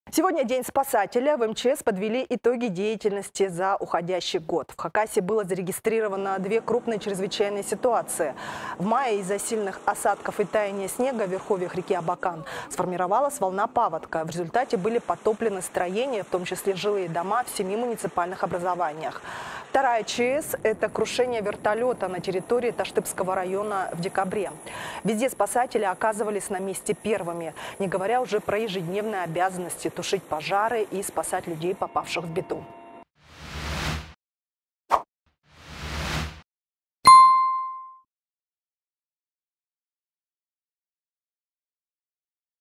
27 декабря - День спасателя. В МЧС Хакасии подвели итоги работы в 2021 году
В Хакасии было зарегистрировано две крупные чрезвычайные ситуации. В мае из-за сильных осадков и таяния снега в верховьях реки Абакан сформировалась волна паводка. В результате были подтоплены строения, в том числе жилые дома в семи муниципальных образованиях. Вторая ЧС - это крушение вертолета на территории Таштыпского района в декабре. Везде спасатели оказывались на месте первыми. Не говоря уже про ежедневные обязанности - тушить пожары и спасать туристов.